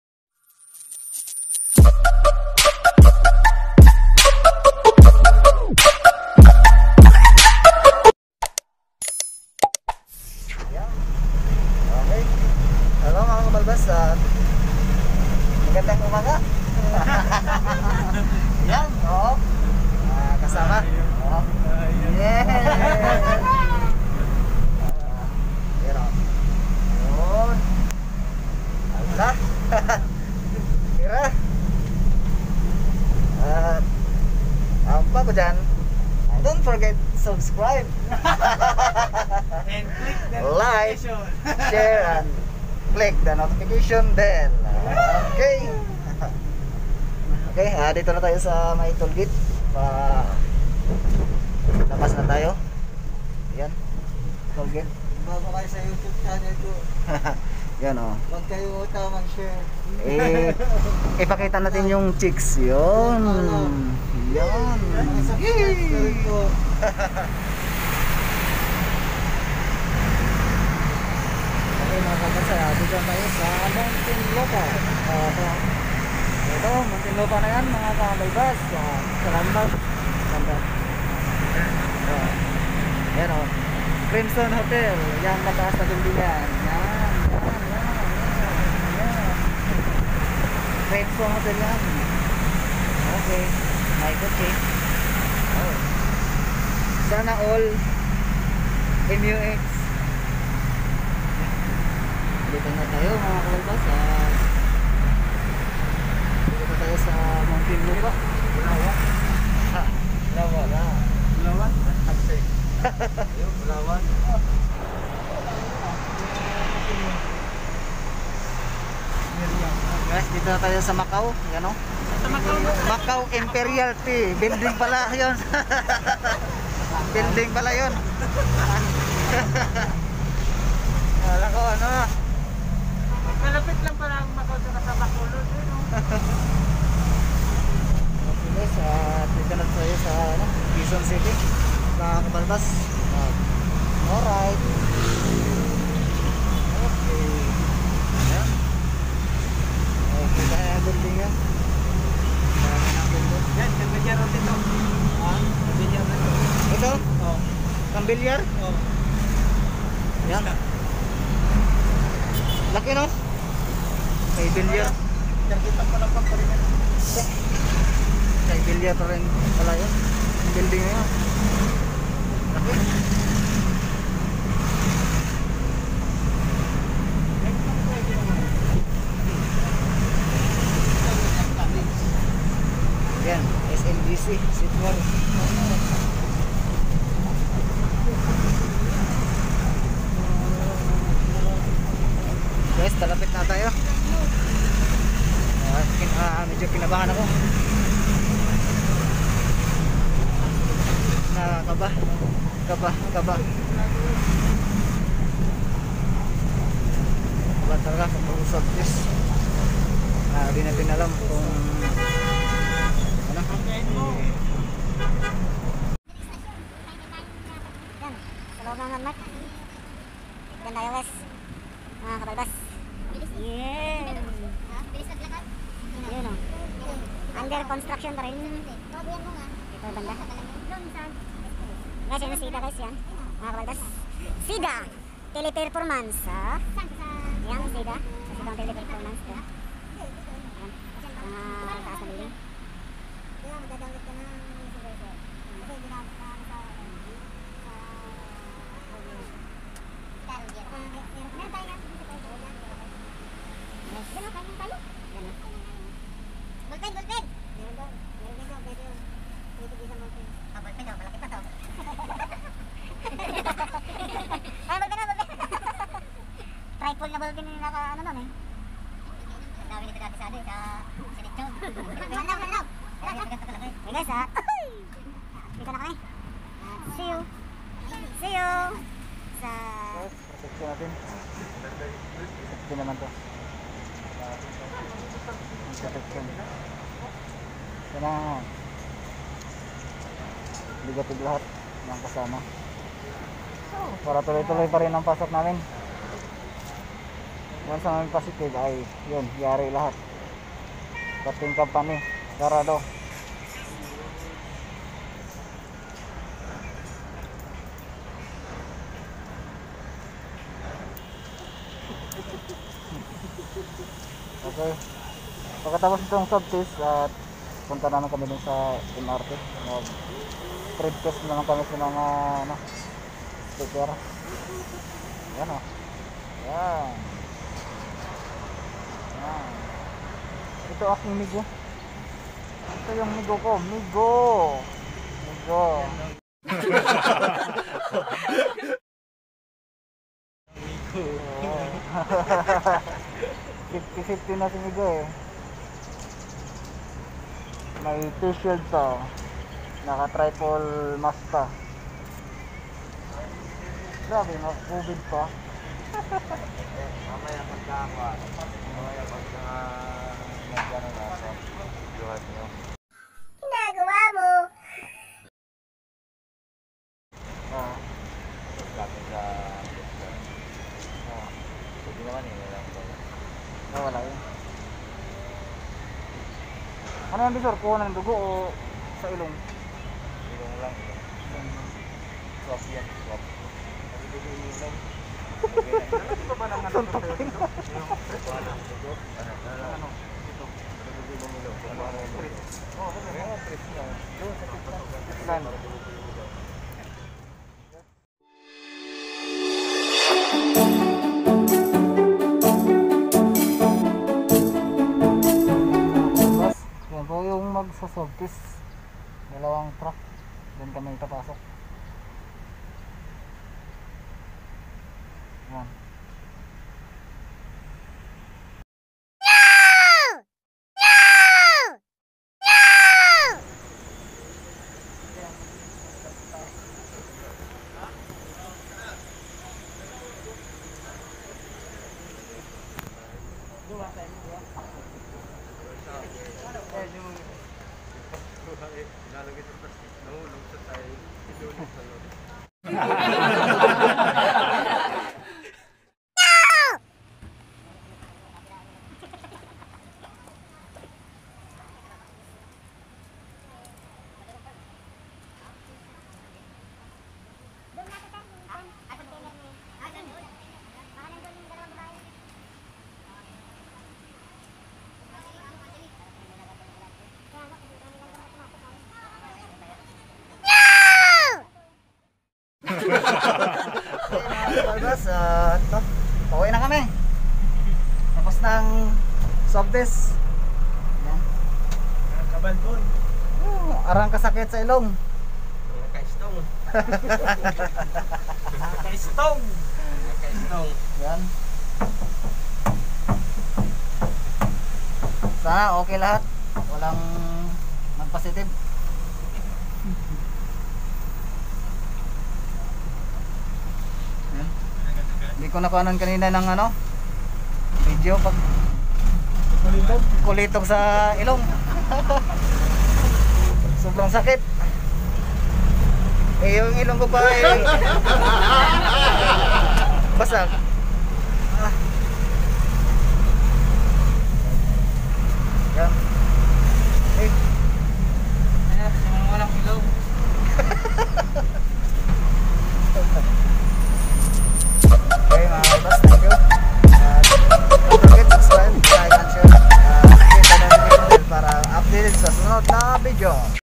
KABALBAS(VLOG) My first swab test..
Going to teroma for swab test.
Kulitan time with co-workers of GFR ENGINEERING.